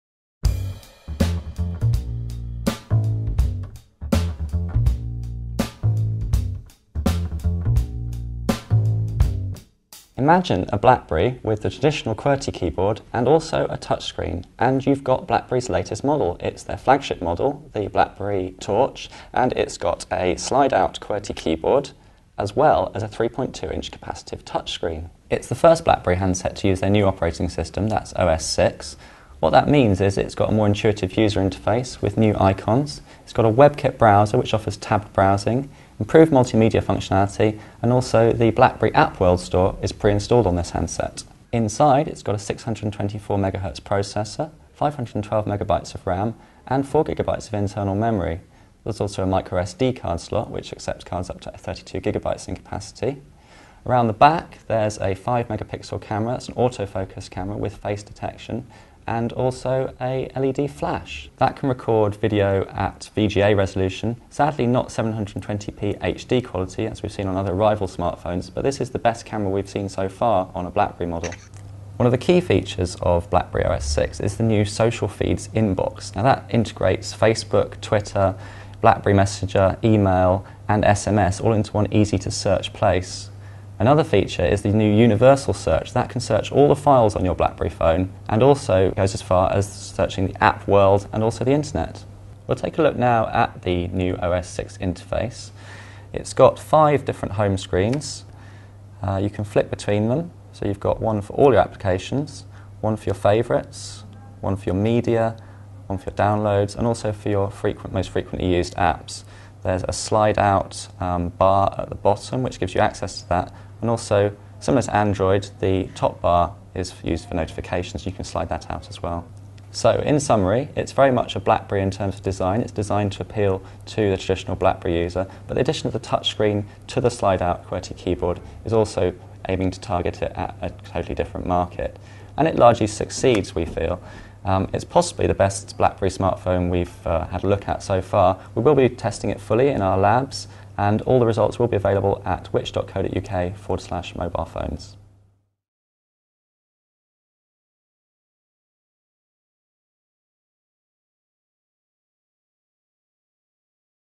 Preview: BlackBerry Torch (Which?)
Wil je meer informatie over mobiele telefoons? Kijk dan op Onze Engelse zusterorganisatie Which? maakte een preview over de BlackBerry Torch.